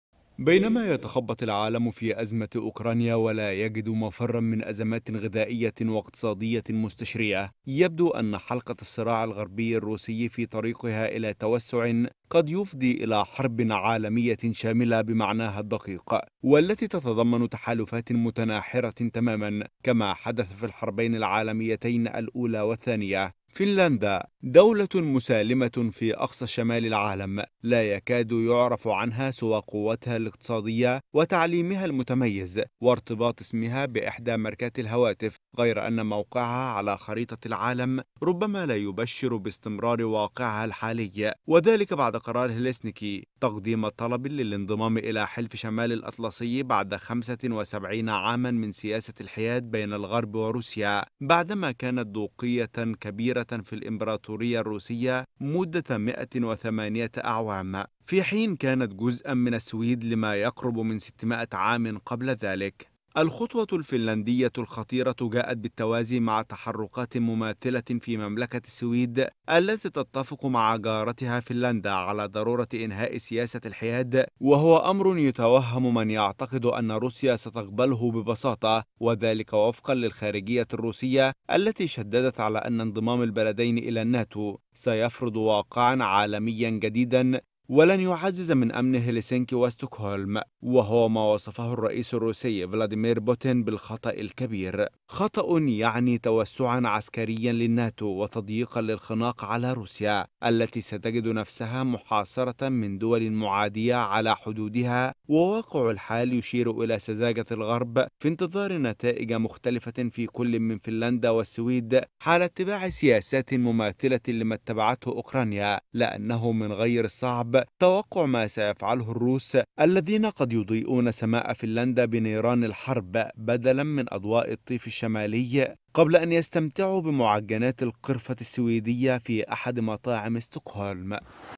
فنلندا والسويد من الحياد إلى الصدام.. هل يصمد الناتو أم يقضم الدب؟
نستعرض لكم خلال التقرير التالي كواليس التوتر الغربي الروسي، وآخر تفاصيل انضمام فنلندا والسويد إلى
أخبار_TeN